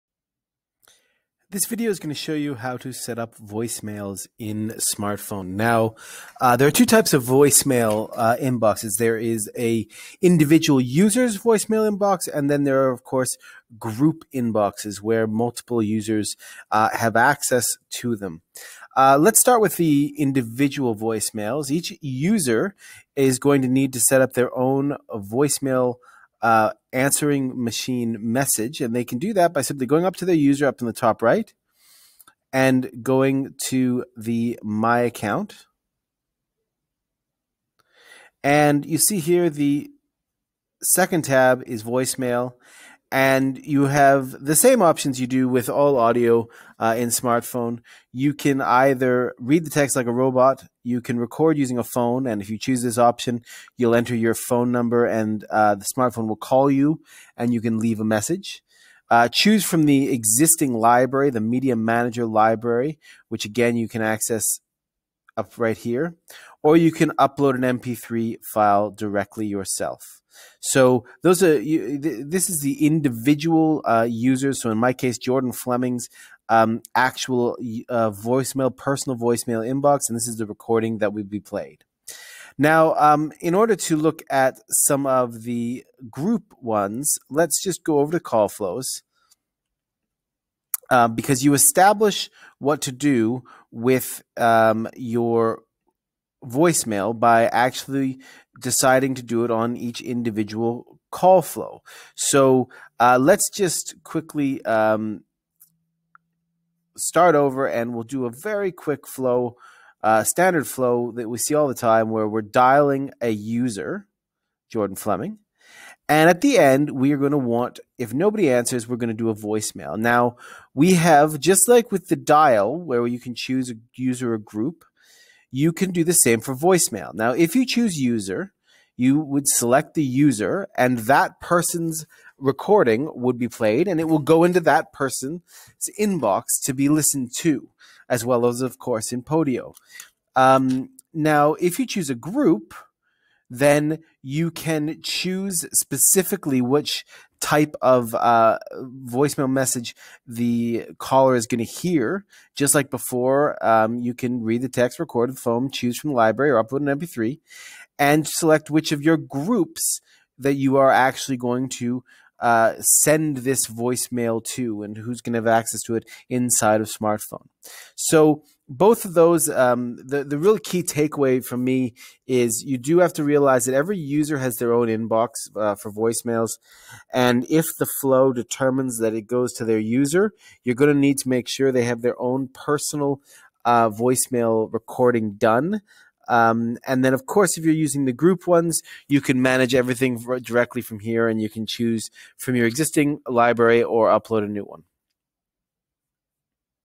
How to set up user & group voicemail in smrtPhone
A quick guide to setting up your outgoing voicemail messages for both individual users and for groups within smrtPhone.